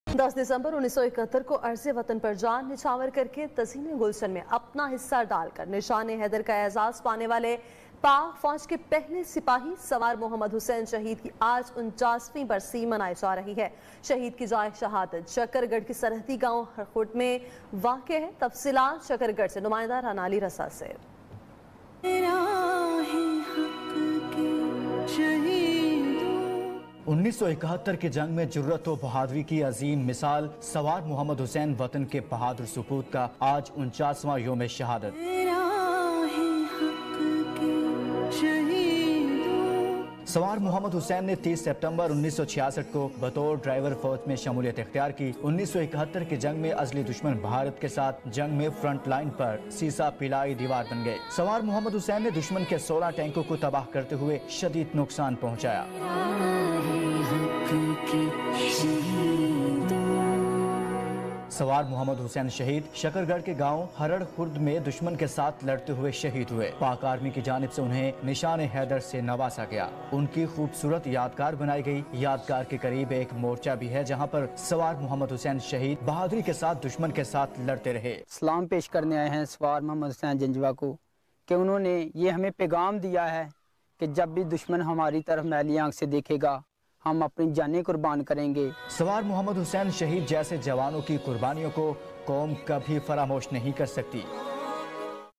Tribute to Swar Muhammad Hussain Shaheed at his 49 Shahadat anniversary!
جائے شہادت مقام ہڑر خورد شکرگڑھ 10 دسمبر 1971 کی جنگ میں دشمن فوج سے لڑتے ہوئے شکرگڑھ ہڑر خورد کے مقام  پر جام شہادت نوش کرنے والے سوار محمد حسین کا 49 واں یوم شہادت آج منایا جارہا ہے
پاک فوج کےعظیم سپوت کو جرات وبہادری کی اعلیٰ مثال قائم کرنے پرنشان حیدرعطا کیا گیا، وہ پہلے سپاہی ہیں جنہیں یہ اعزاز ملا۔سوارمحمد حسین شہید 18 جون 1949 کو دولتالہ کے قریب ڈھوک پیر بخش میں پیدا ہوئے، سترہ برس کی عمر میں انھوں نے پاکستان آرمی میں ڈرائیورکی حیثیت سے شمولیت اختیارکی اور ڈرائیور ہونے کے باوجود 1971 کی عملی جنگ میں بھر پور حصہ لیا.1971کی جنگ میں سیالکوٹ میں ظفر وال اور شکر گڑھ کے محاذ جنگ پر مسلسل پانچ دن تک دشمن کی گولہ باری کے باوجود اگلے مورچوں تک اسلحہ پہنچاتے رہے
 ان کی نشاندہی پر ہی پاک فوج نے ہندوستان کی فوج کے 16 ٹینک تباہ کیے۔۔۔۔۔۔۔